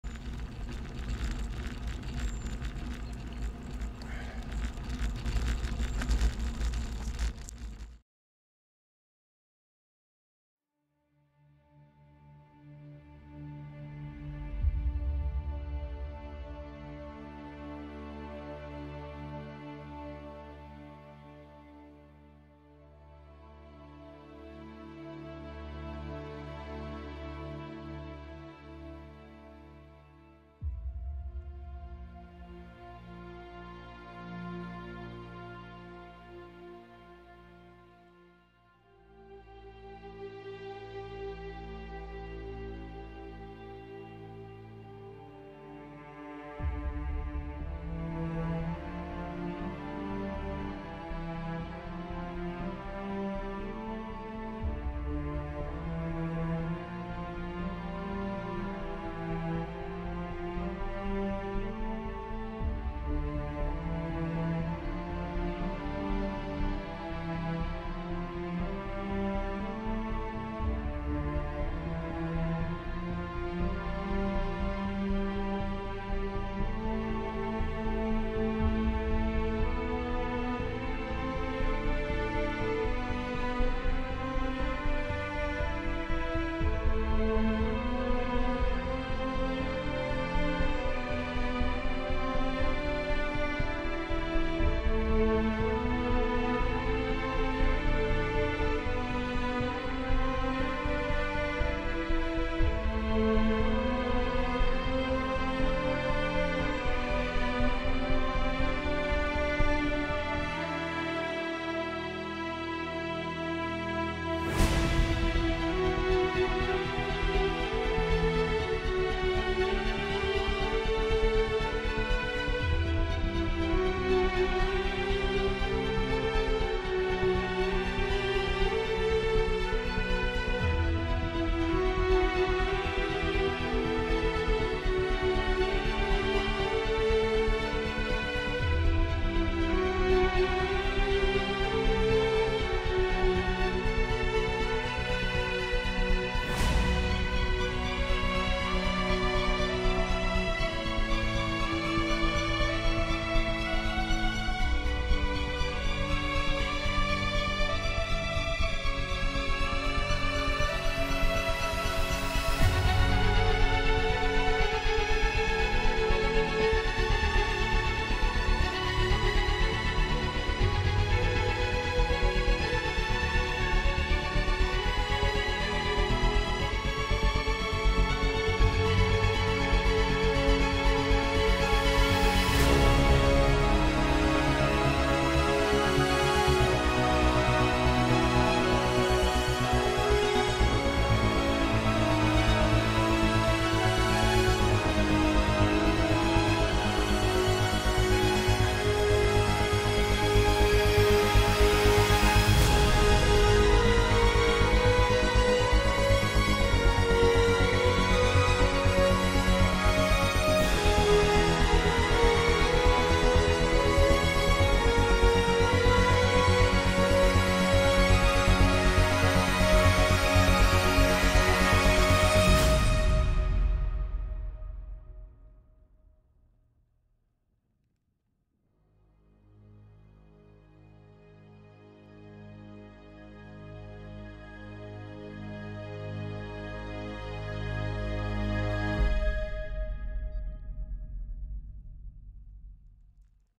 Bordeaux - Genève 2023: Dag 11 Chezéry-Forens naar St.Étienne-du-Bois
De laatste officiële etappe van deze vakantie. Met mooi uitzicht vanaf Belvédère de Catray vlak voor Col de Cuvéry. Bij de brug bij Thoirette over L'Ain. Om vervolgens uit te komen bij de laatste camping in Saint-Étienne-du-Bois.